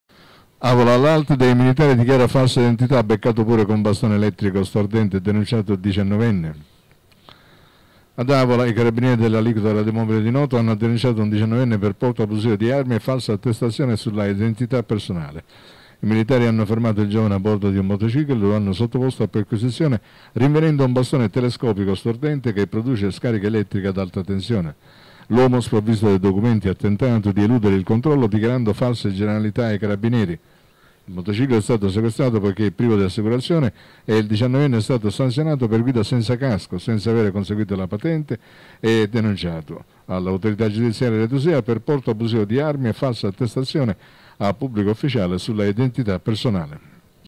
Avola. All’alt dei militari dichiara falsa identità, beccato pure con bastone elettrico stordente
Ad Avola, i Carabinieri dell’Aliquota Radiomobile di Noto hanno denunciato un 19enne per porto abusivo di armi e falsa attestazione sull’identità personale.

I militari hanno fermato il giovane a bordo di un motociclo e lo hanno sottoposto a perquisizione, rinvenendo un bastone telescopico stordente che produce scariche elettriche ad alta tensione.

L’uomo, sprovvisto di documenti, ha tentato di eludere il controllo dichiarando false generalità ai Carabinieri.

Il motociclo è stato sequestrato poiché privo di assicurazione e il 19enne è stato sanzionato per guida senza casco, senza aver conseguito la patente e denunciato all’Autorità giudiziaria aretusea per porto abusivo di armi e falsa attestazione a Pubblico Ufficiale sull’identità personale.